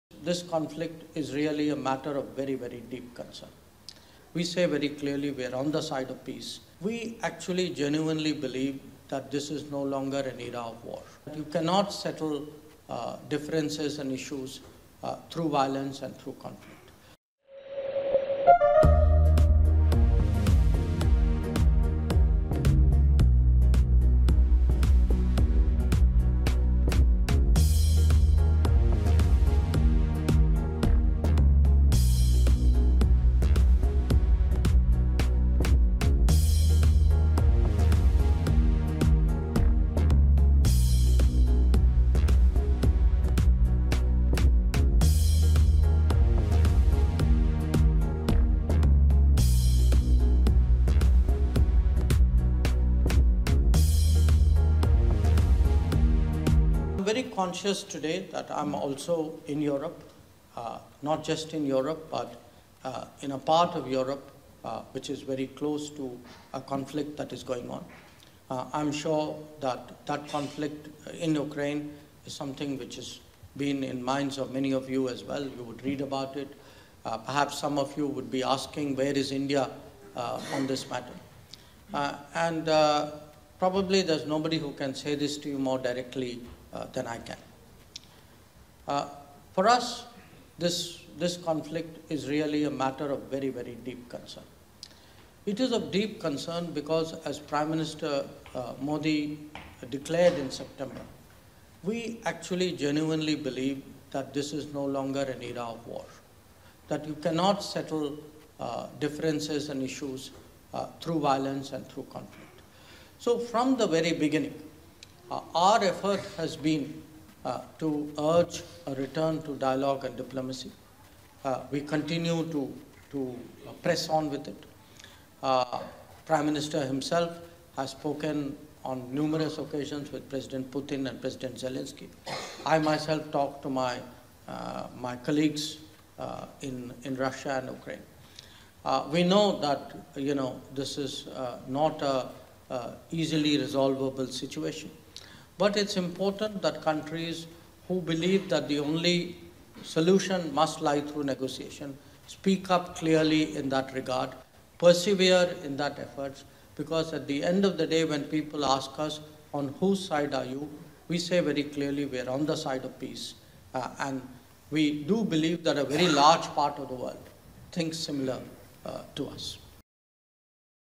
“India On Side Of Peace”: EAM Jaishankar On Russia-Ukraine Conflict
External Affairs Minister Dr S Jaishankar addressed Indian Diaspora in Vienna, Austria. While addressing the event he said, “For us, (Russia-Ukraine) conflict is a matter of deep concern. We believe this is not an era of war. From the beginning, our effort has been to return to dialogue & diplomacy. We're on the side of peace and a large part of the world thinks like us.”